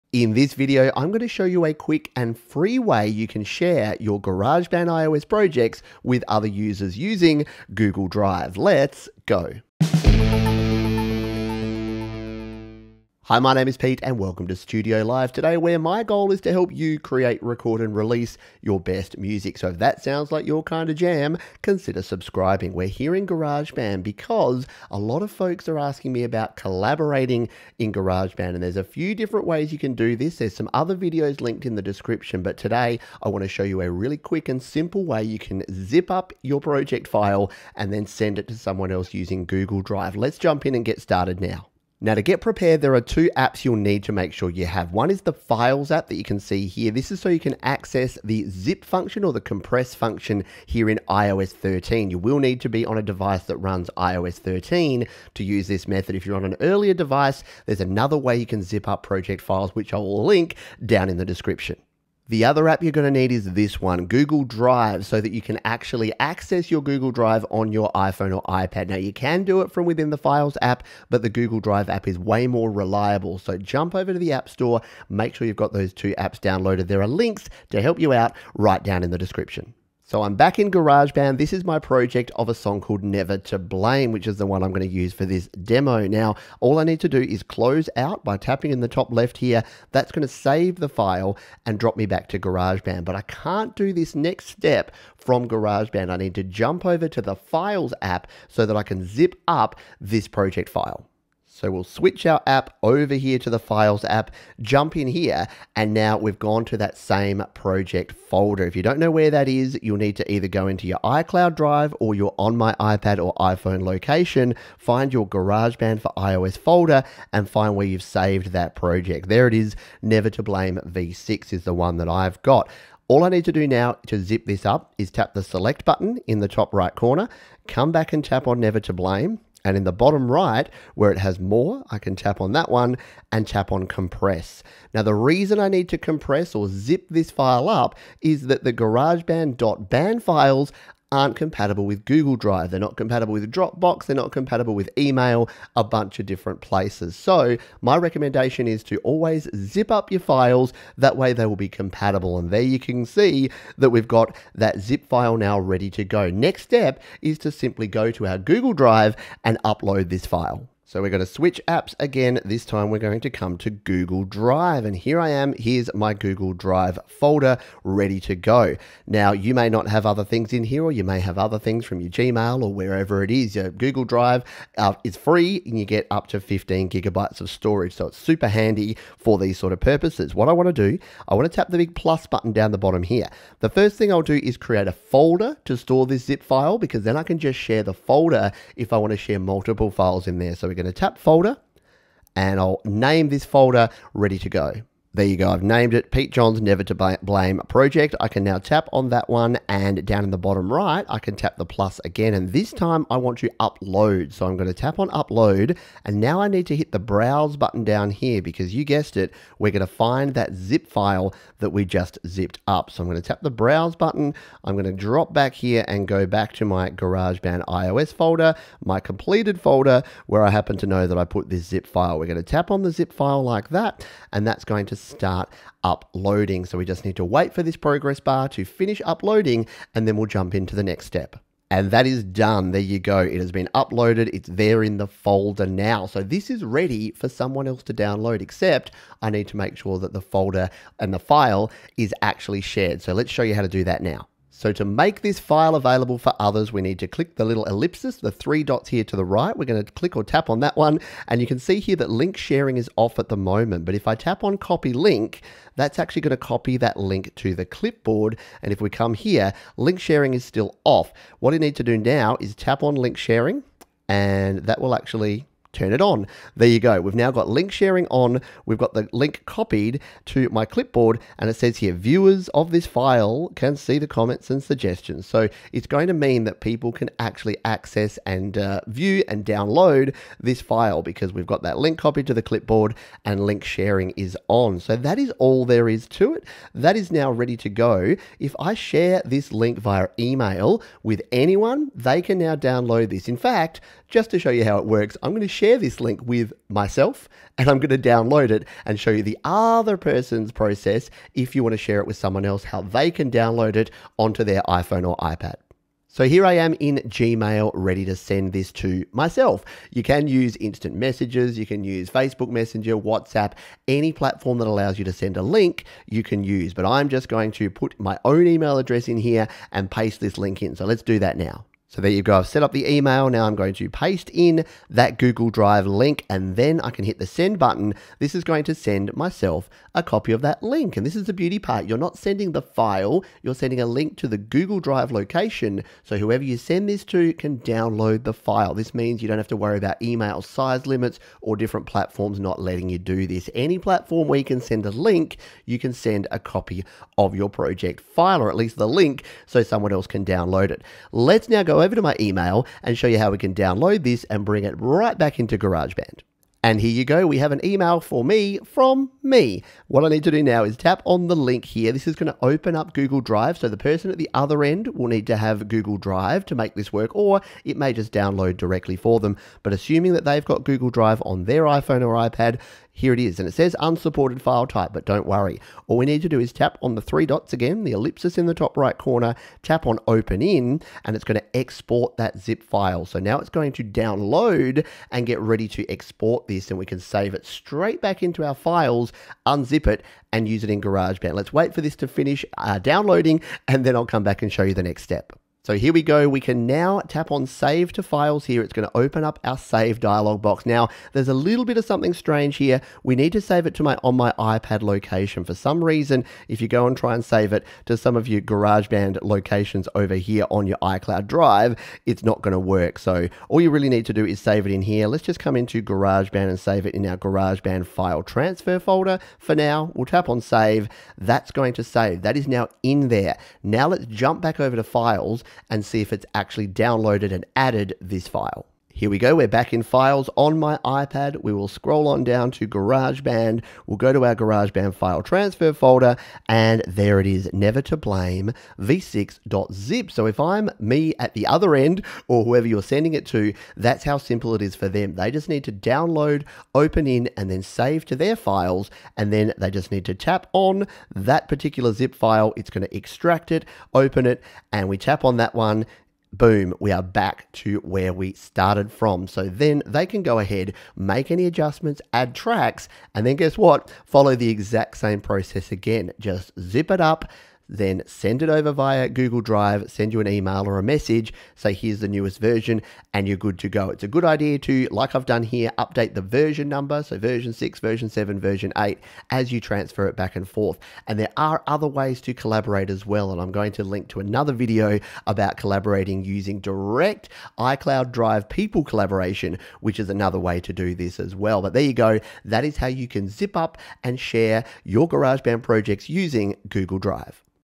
How to SHARE GarageBand iOS projects with GOOGLE DRIVE
How to use GOOGLE DRIVE to share your GarageBand iPad and GarageBand iPhone projects, by using the compression (zip) function in iOS 13.

⏰ Time Stamps:
00:00 Introduction
00:42 What apps you will need (Files and Google Drive)
01:20 Saving a GarageBand iOS project
01:42 Compress (ZIP) a GarageBand iOS project
02:14 Why you need to compress GarageBand .band project files
02:40 Upload a GarageBand iOS project using Google Drive
03:59 How to share a Google Drive file or folder
05:05 How to send a Google Drive link to another user
05:57 How to download a file from Google Drive
06:34 How to save a downloaded GarageBand project
07:20 How to open a downloaded GarageBand project